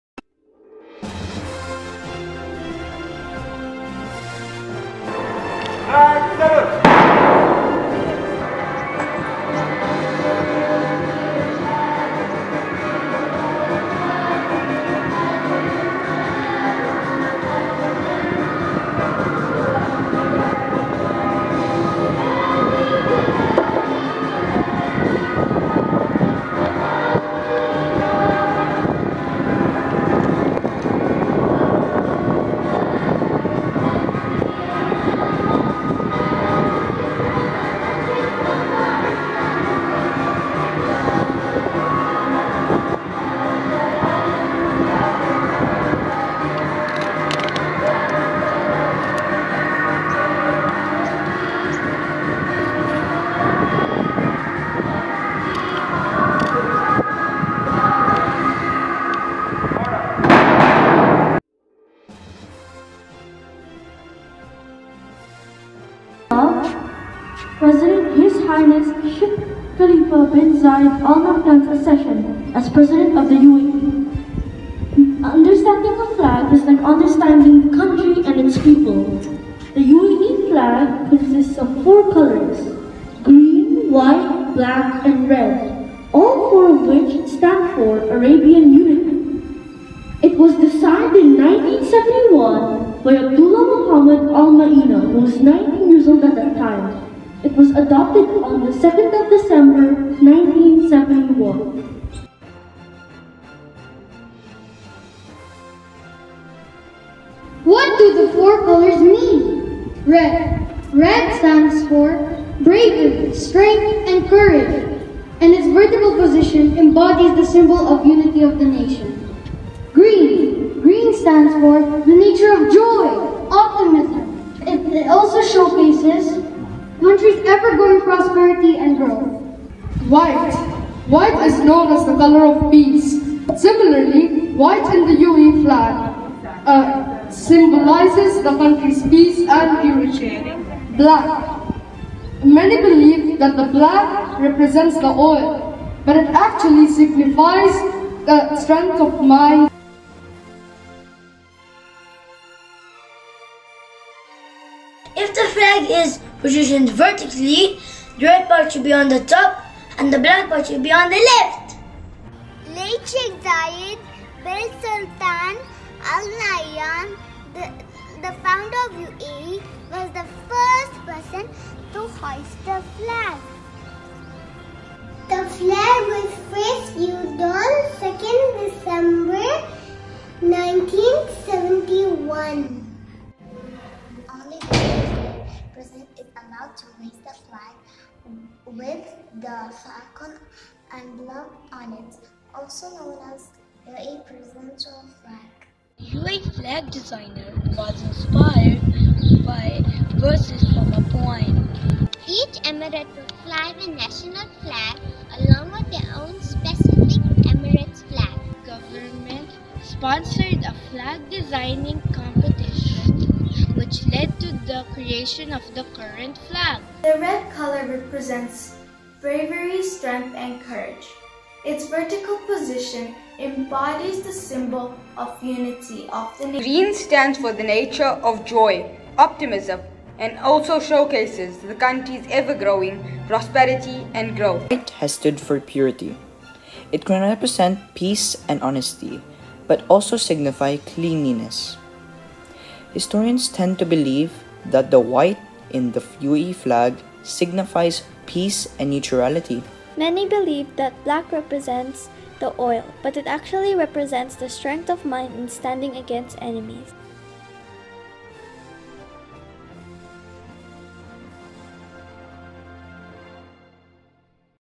Zakher Private School British Division Celebrates UAE Flag Day 2020
The United Arab Emirates (U.A.E) Flag Day is a national occasion where people of the UAE remember the efforts of the founders of their State, Sheikh Zayed and Sheikh Rashid, and their brothers who sacrificed everything for the sake of their nation. It is a day when the national spirit is renewed, and an occasion to reflect on the achievements of the era of President Sheikh Khalifa bin Zayed Al Nahyan.